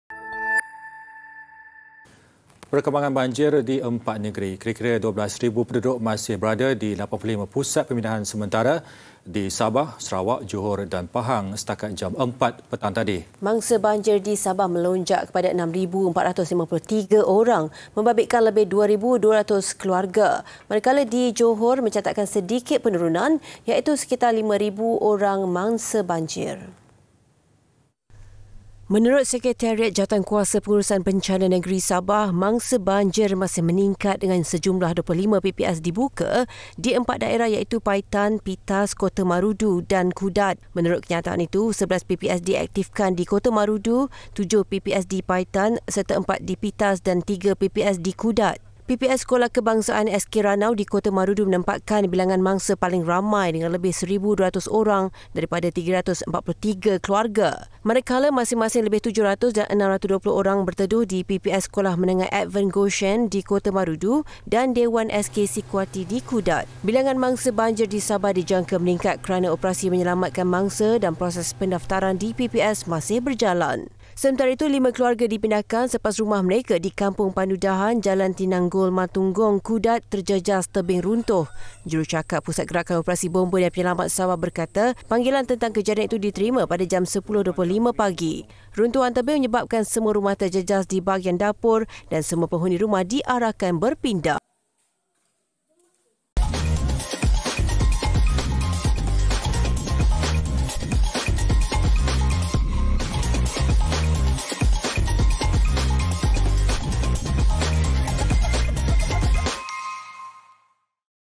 BANJIR | Hampir 12,000 Mangsa Banjir Di 4 Negeri
Kira-kira 12 ribu penduduk masih berada di 85 Pusat Pemindahan Sementara (PPS) di Sabah, Sarawak, Johor dan Pahang setakat jam 4 petang tadi.

Mangsa banjir di Sabah melonjak kepada 6,453 orang membabitkan lebih 2,200 keluarga.

 Manakala di Johor mencatatkan sedikit penurunan iaitu sekitar 5,000 orang mangsa banjir.